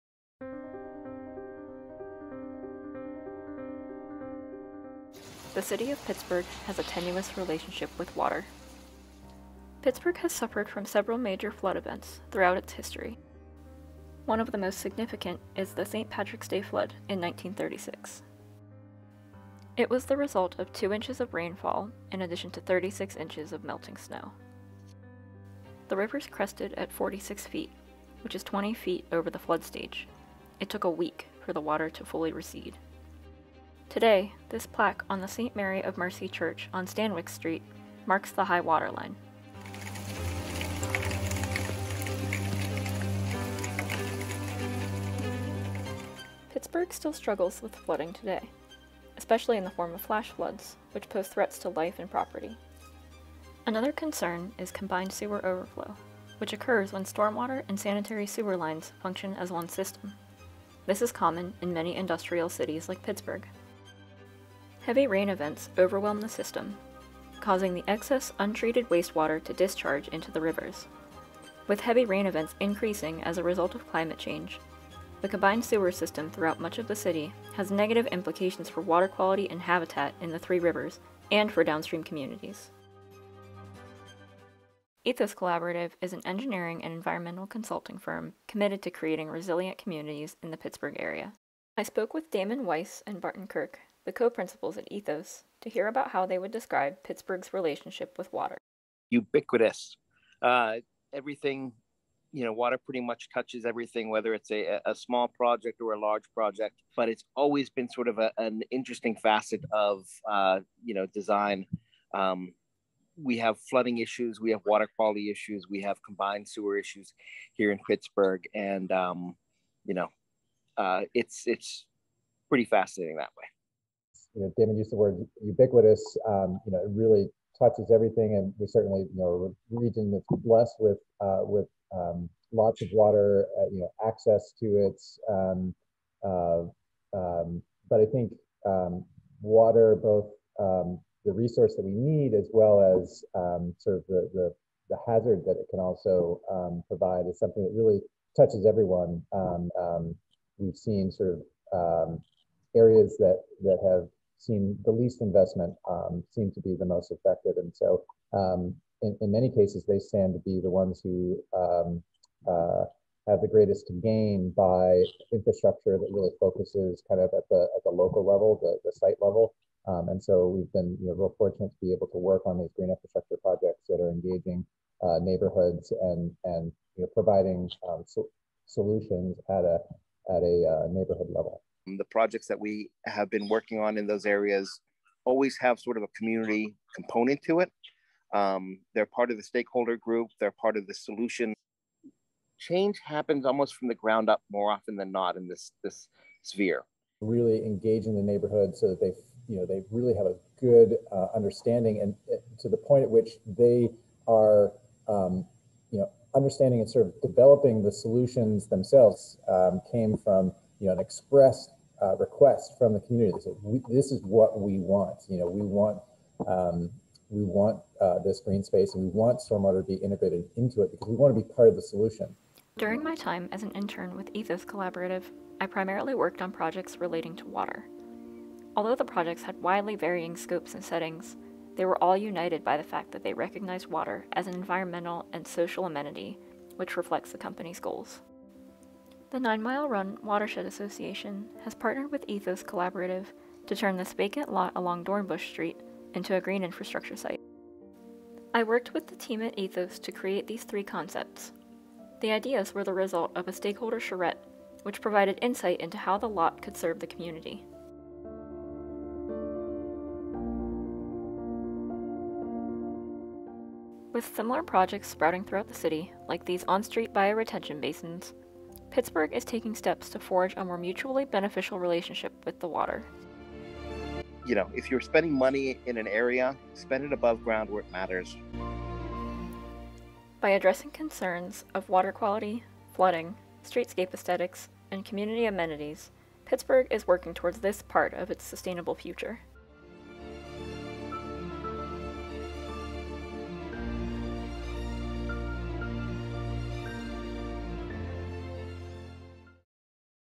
Spring 2021 City Semester: Madison Borsos
Major: Landscape Architecture
Internship: Ethos Collaborative
During her internship with Ethos Collaborative, Madison learned about Pittsburgh’s history of floods and its ubiquitous relationship with water. She worked with the team on various projects that engage neighbors to be a part of the solution in creating resilient communities. Most of the projects Madison worked on related to water, including a collaboration with Nine Mile Run Watershed Association to develop design concepts to turn a vacant lot into a green infrastructure site based on stakeholder feedback.